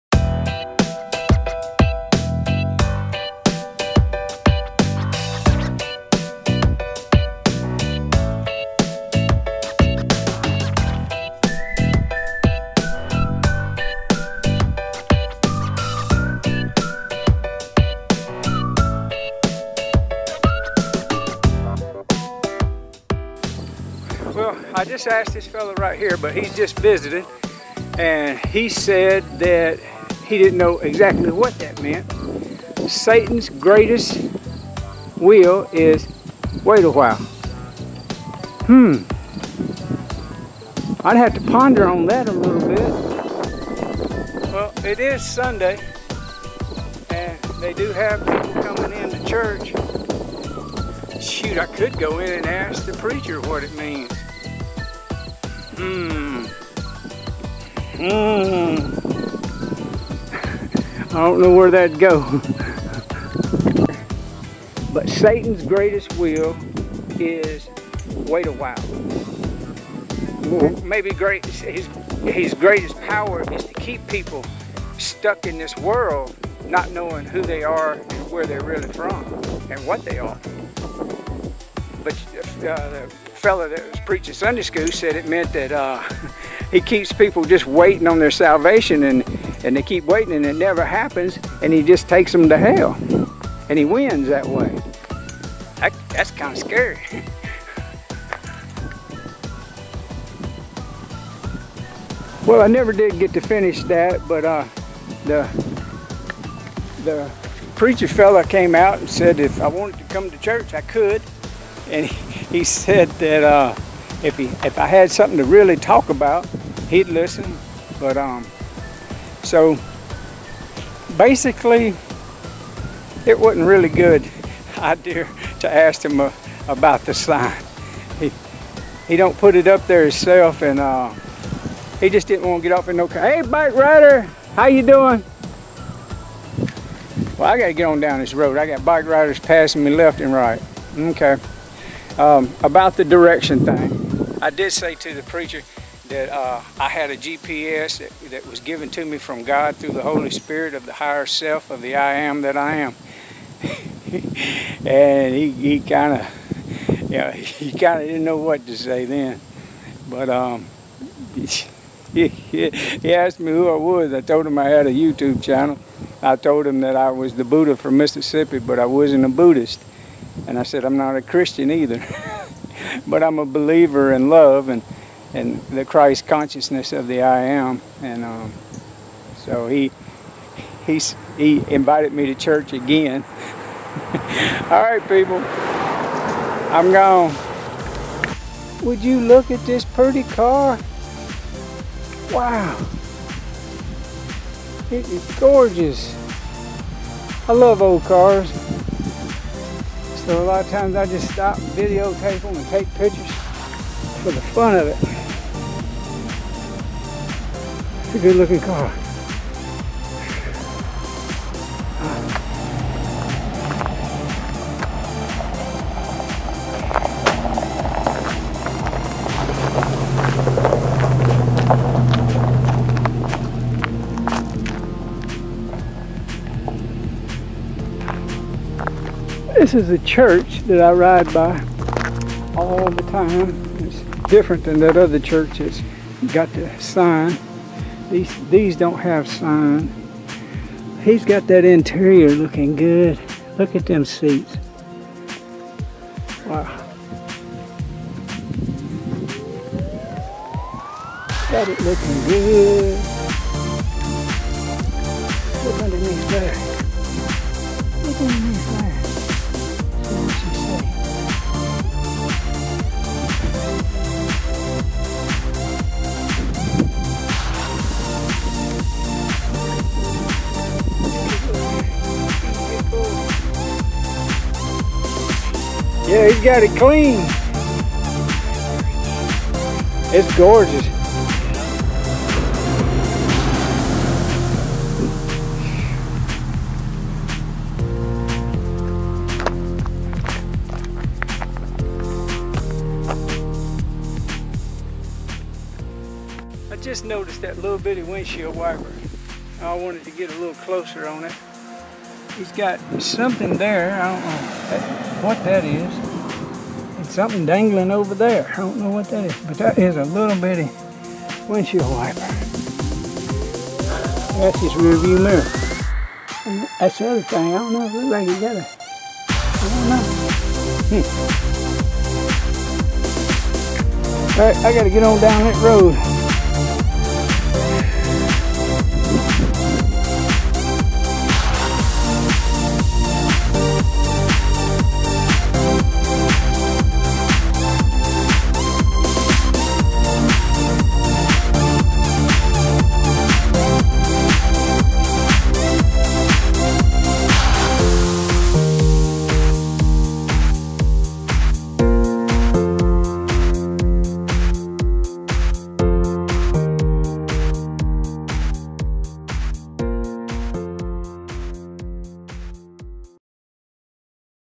What I saw on my 62.9 mile Bike Ride 08-26-18
What I saw on my Bike Ride 08-26-18

~ Buddha   : )